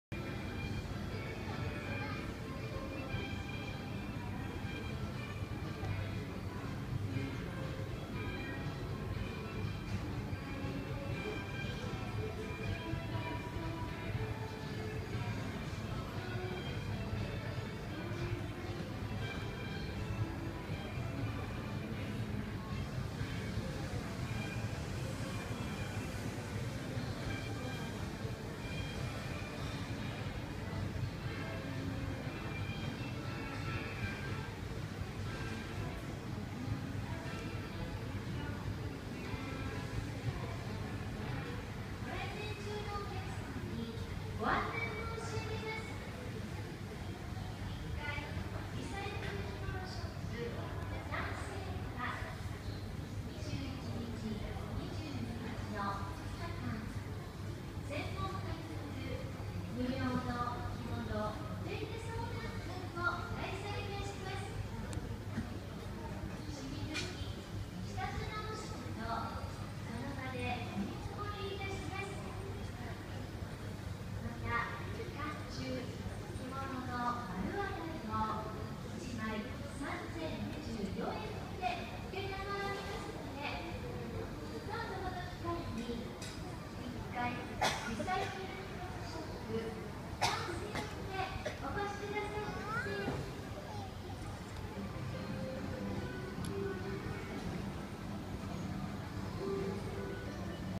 インディアン モーレイ
エルム水族館で、一番大きな魚ちゃんです。インド洋とか、インドネシア周辺に住んでいる魚ちゃんらしいです。

大きくなると、150センチ以上にも
なるらしい〜

いつも、動かないで、じーと横にいるの
しか、見た事がないのに、
今日は、動いているのを、初めて見ましたよ〜  ♪

全部で、３匹いました。
きっと、お値段、高いだろうなぁ〜
などと、主婦の私は思いました。

館内放送が、入ってしまいました。
そのあと、撮影しようと、したら
また、動かないで、じっとしたまま
でした。撮影は、昼過ぎです。